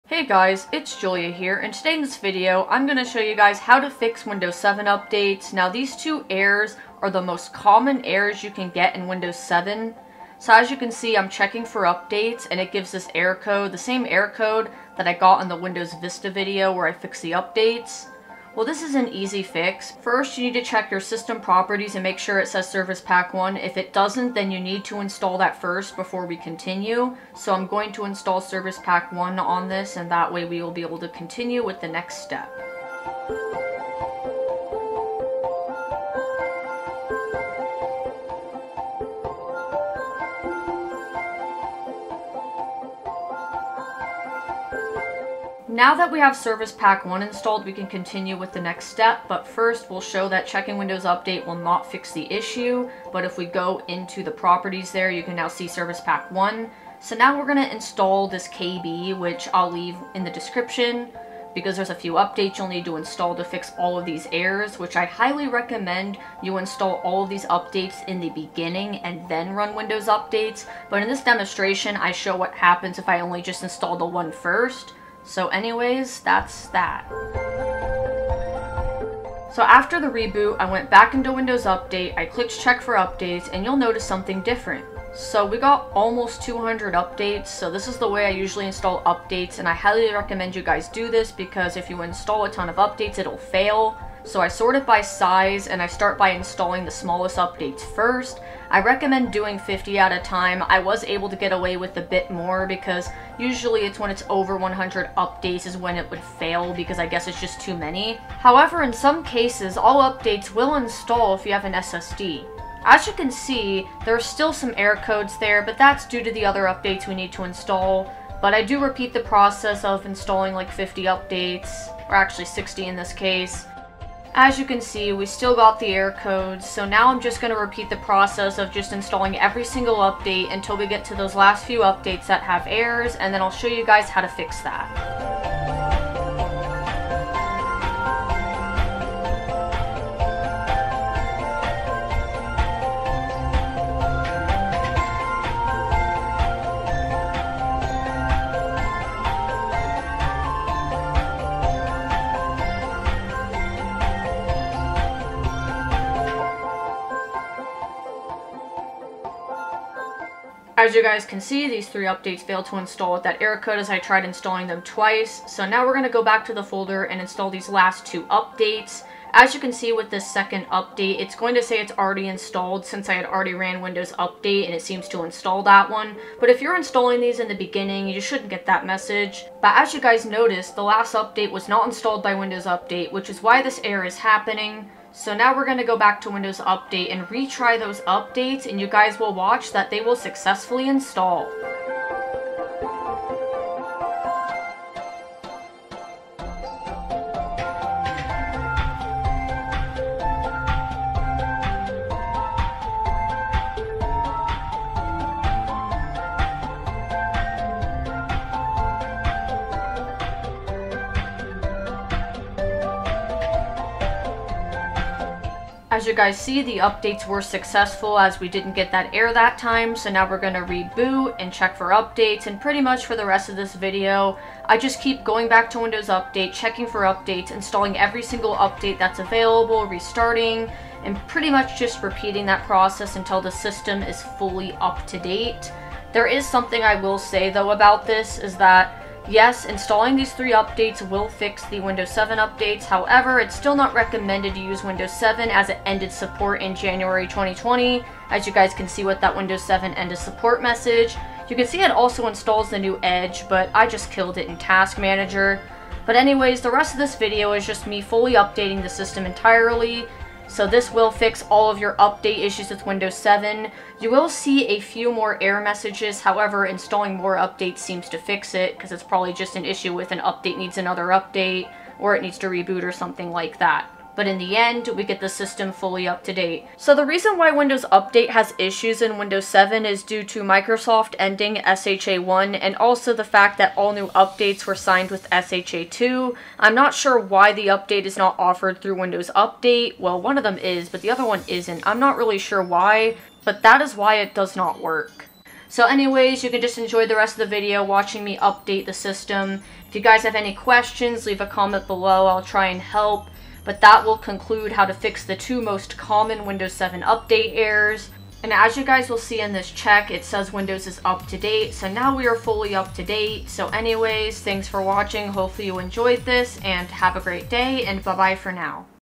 Fix common Windows 7 update errors (80072EFE & 80092004)
I show how to fix the two most common Windows 7 update errors, which are error codes 80072EFE and 80092004. Note: You must have Windows 7 SP1 installed before installing these updates. I highly recommend installing all 3 updates then reboot it (no need to reboot after each one). After installing those 3 updates, you shouldn't run into any issues.

If you get another error, that will be fixed later on by checking for updates and installing more updates. You will know it's up to date when you click check for updates and it says it's up to date. I show my entire update process on a Windows 7 computer.

I do not recommend using Windows 7 as a main OS, as it ended support in January 2020. It will only update as far as January 2020. I show how to fix these in case you have an old Windows 7 computer laying around, or want to mess with it. Windows 7 works best with all the updates.

Link to updates:
Windows 7 SP1 update (in case you don't have it installed)

Chapters:
0:00 - Introduction & showing error 80072EFE
0:19 - Checking system properties for SP1
0:25 - Installing Windows 7 SP1
0:45 - Showing error & system properties again
0:57 - Installing KB3138612
1:19 - Checking for updates in WU
1:25 - WU found updates / How I install updates
1:48 - Installing updates
2:40 - Got error 80092004
2:47 - Installing the last 2 updates needed to fix WU
3:07 - Retrying the failed updates
3:35 - Updates successful
3:51 - Installing more updates in WU
4:03 - Talking about Windows 7 end of support
4:17 - Fully updating the system
4:36 - Why WU is so broken in Windows 7
4:55 - Wrap up
5:09 - Windows is up to date